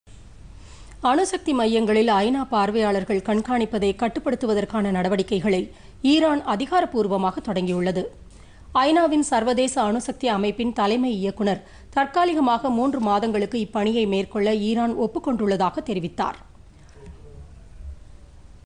ஐ.நா பார்வையாளர்களை கட்டுப்படுத்த முயற்சி
அணுசக்தி  மையங்களில் ஐநா பார்வையாளர்களின் கண்காணிப்பை கட்டுப்படுத்துவதற்கான நடவடிக்கை -  ஈரான் அரசு அதிகாரப்பூர்வமாக தொடங்கியுள்ளது.